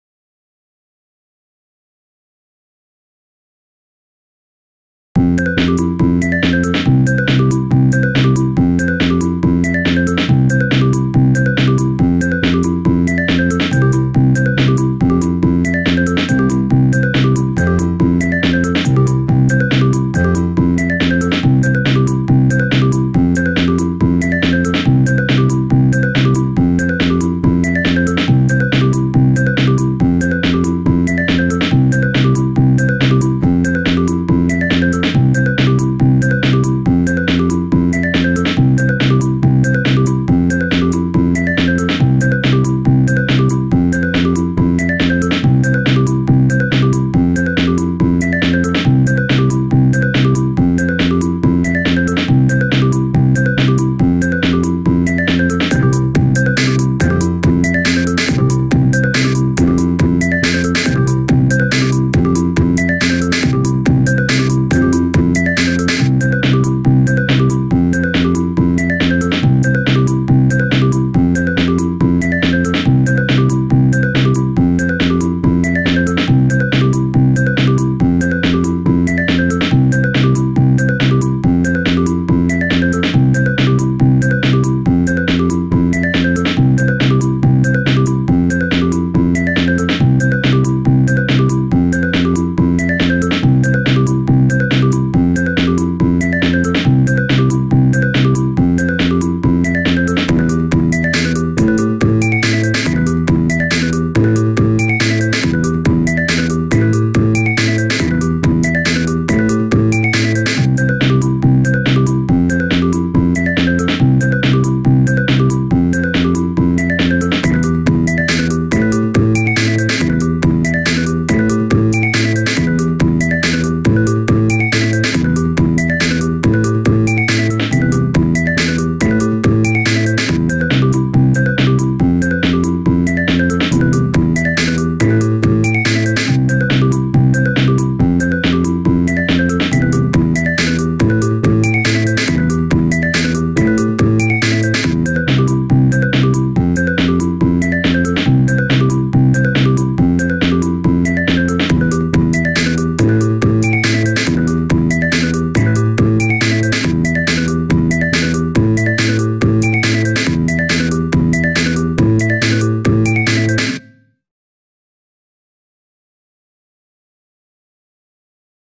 Using the "when" statement to change time signature in FoxDot
A demonstration of a function I've been working on for a while (the "when" statement) for Live Coding music. It show how you can change all the indivuidual FoxDot Players in your piece of music from 4/4 to 3/4 and back with a few lines of code.